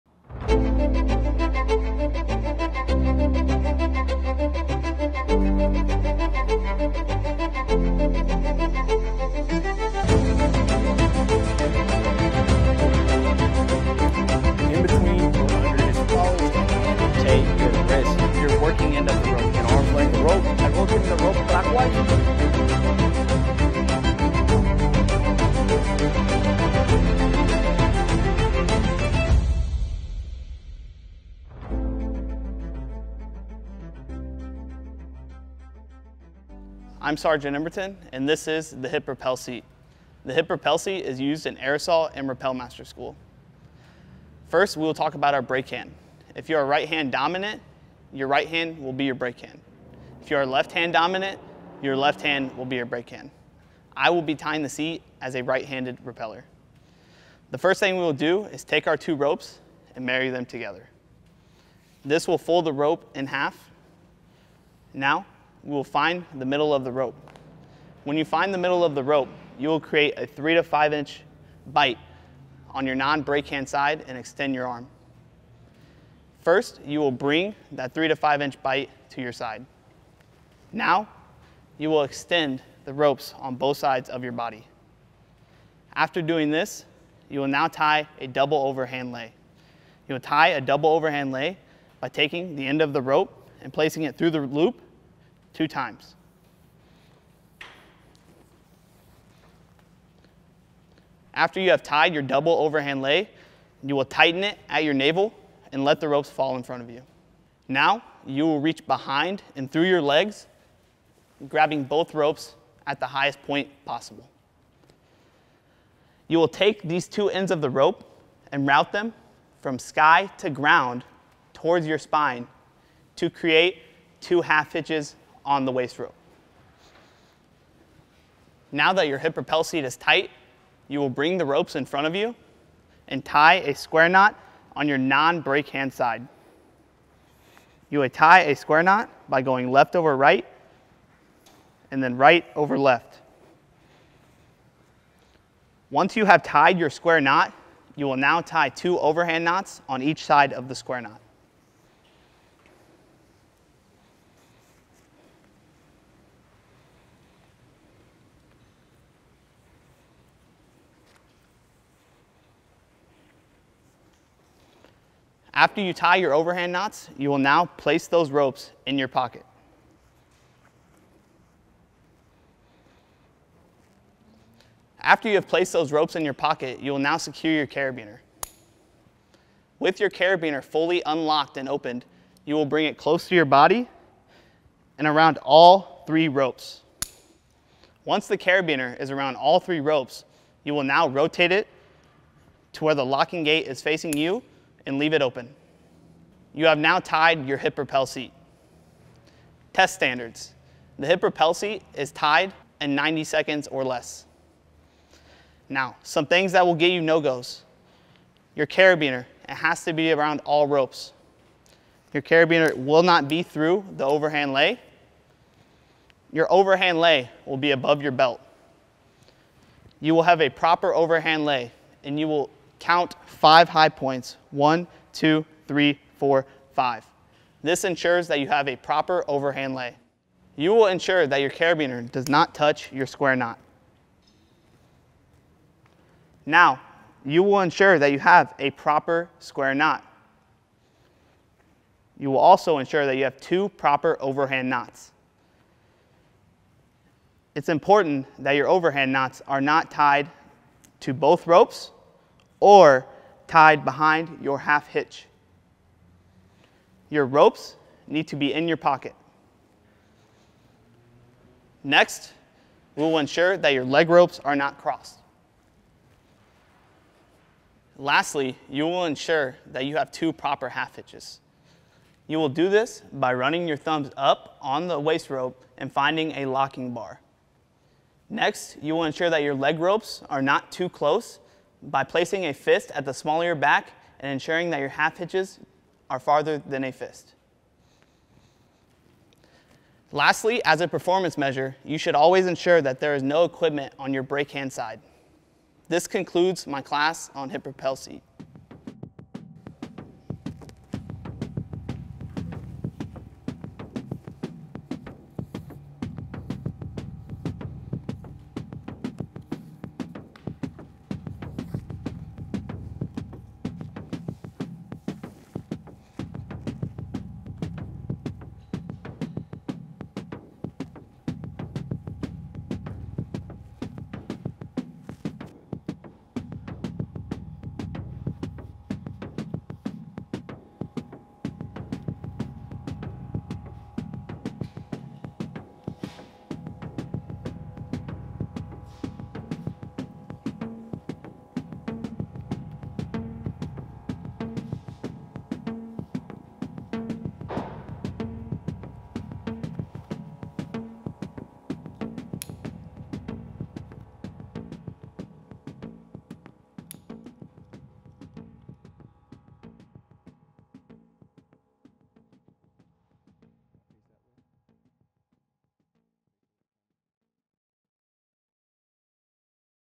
Hip Rappel Seat (Rappel Master)
Testable Knots of the Rappel Master Course, Schofield Barracks, Hawaii.

Lightning Academy, 25th Infantry Division, US Army.